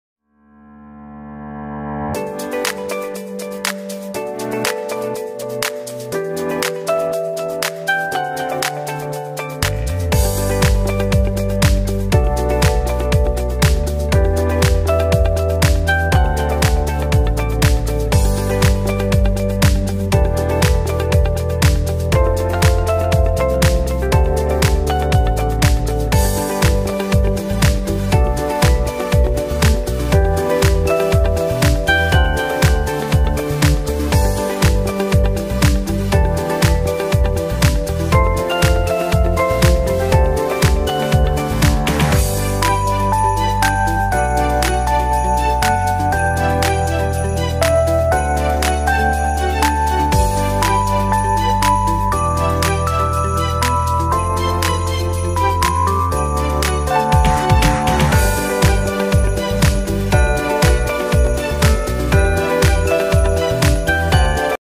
In my bag i find it my cute cat say Happy new year# 2022#cute
If you enjoy this video make sure to give this video a big thumbs up and share it with your favorite friends and family members